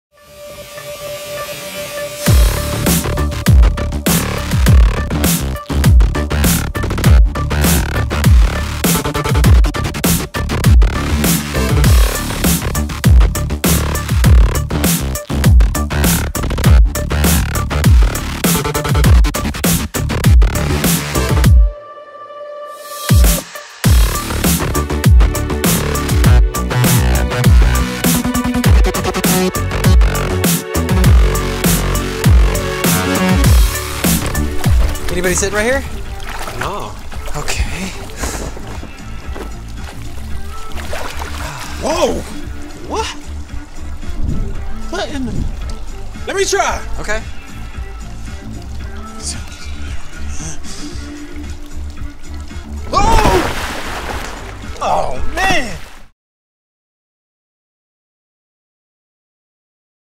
Showreel 2020 - Nacho Hoyos
Algunos de mis mejores trabajos en los últimos años como compositor digital.